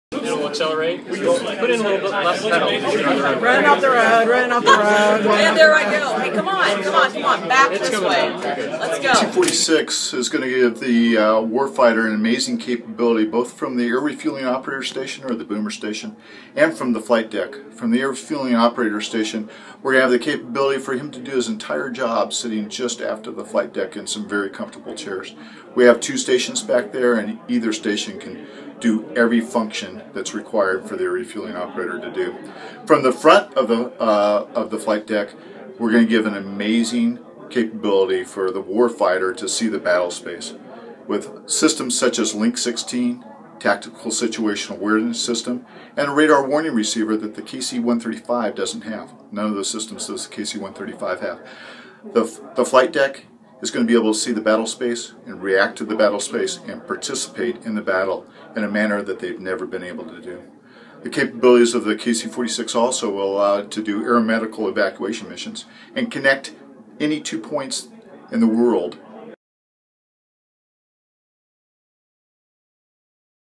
KC-46A Tanker Demonstrator
McConnell Air Force Base tanker crew members get their first look at the new KC-46A through a demonstrator. (Video by Rick Plumlee, Nov. 14, 2013)